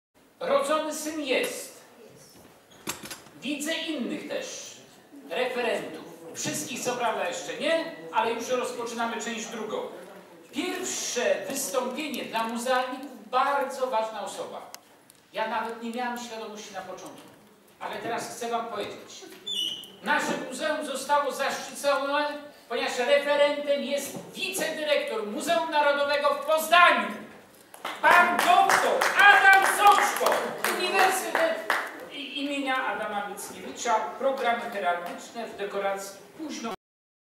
VI KONFERENCJA NAUKOWA W CYKLU SPOTKANIE MALBORSKIE IM.MACIEJA KILARSKIEGO - 10.09.2014 CZ V
W Karwanie odbyła się VI konferencja "Spotkań Malborskich, im. Macieja Kilarskiego". Poświęcona była
ikonografii drzwi i wybranych programach ideowych średniowiecza. W problematykę debaty włączone były kwestie religijne, jak również artystyczne czy literackich przejawów religijnych. Członkowie konferencji przedstawili swoje referaty, a po nich nastąpiła debata oraz podsumowanie poszczególnych z referatów.